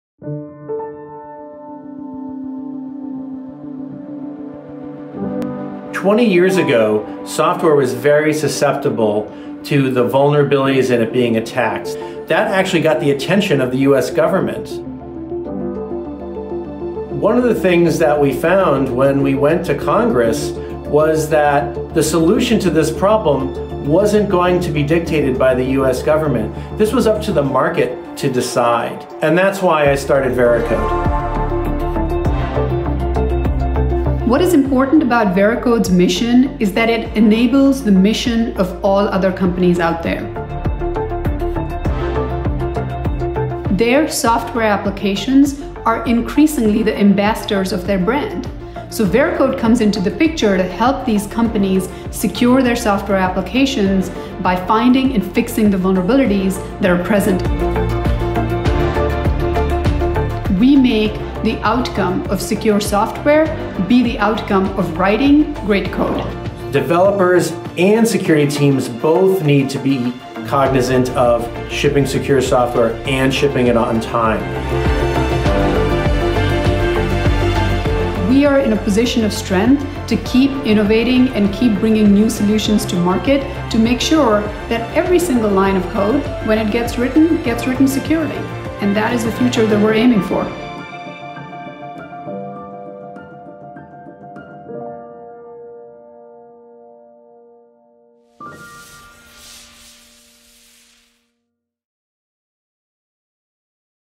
Veracode Corporate Video
Veracode is a SaaS-based application security (AppSec) company founded in 2006 by a group of skilled computer hackers. The hackers, including Chris Wysopal, realized that software was very vulnerable to cyberattacks. Over the years, Veracode has established industry-leading technology that helps developers and security professionals find and fix vulnerabilities in their software. Together with Veracode, customers can confidently secure their code.